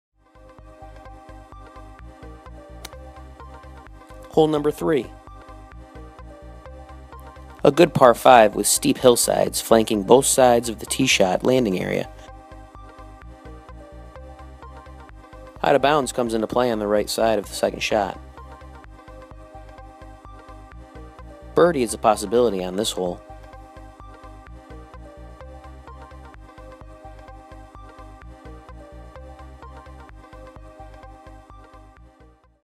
Bull Valley CC Hole #3 Final
Bull Valley Country Club Hole #3 Analysis by Course Pro Mike Picciano. Hole #3 Hillside Par 5 557 Yards